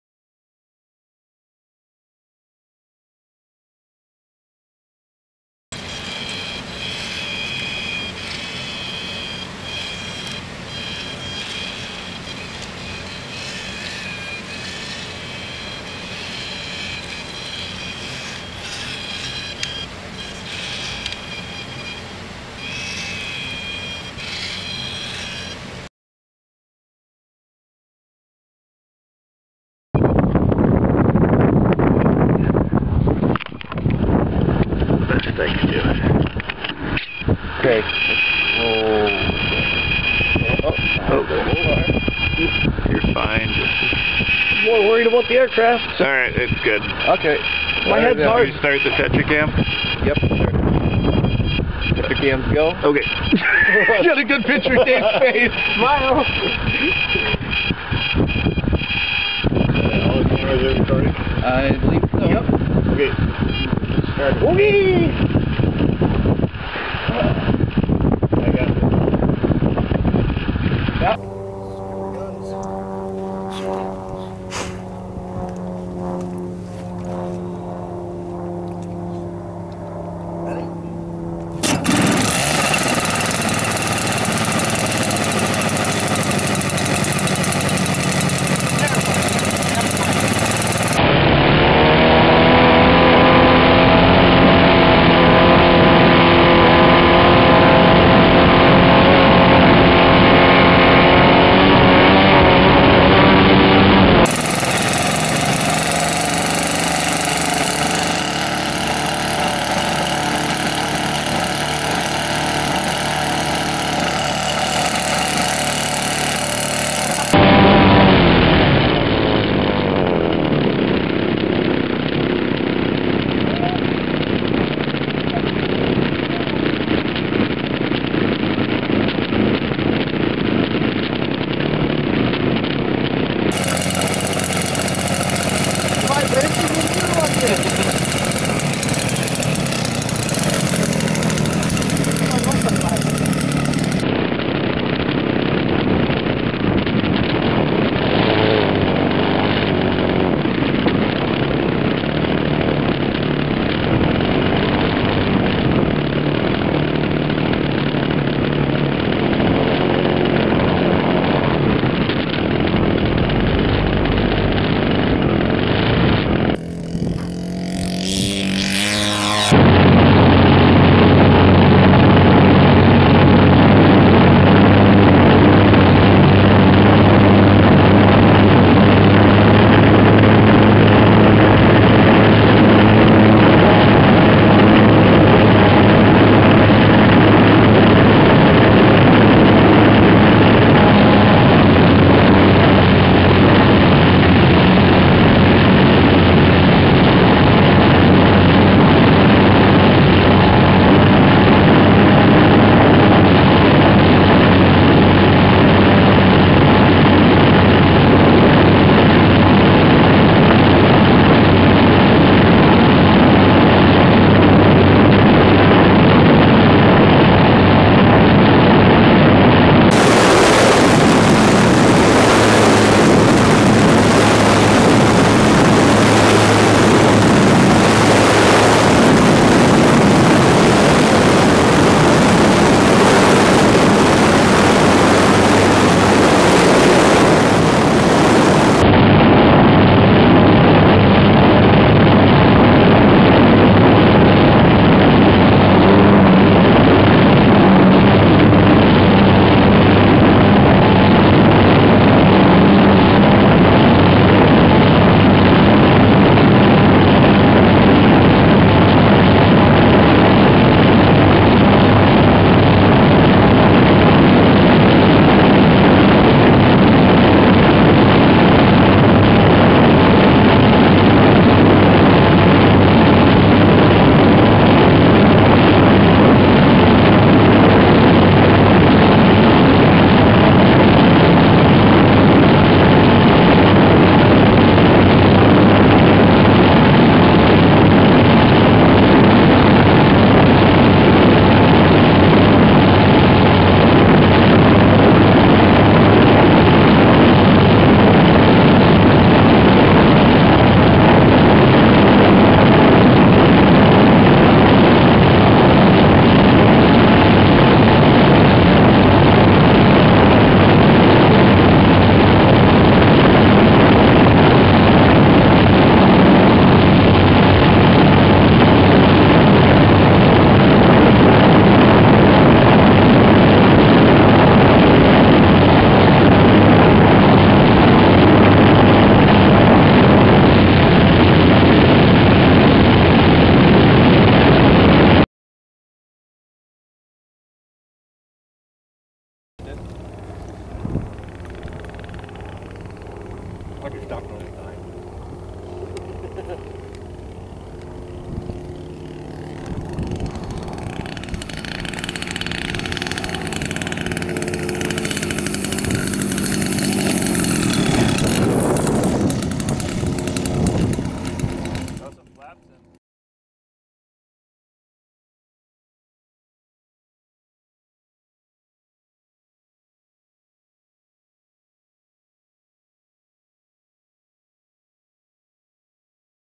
IMU-Based Tetracam Stabilization Flight Test
A Crista IMU, PIC32, and RC-grade servos were used to attempt to stabilize an aircraft mounted tri-band camera.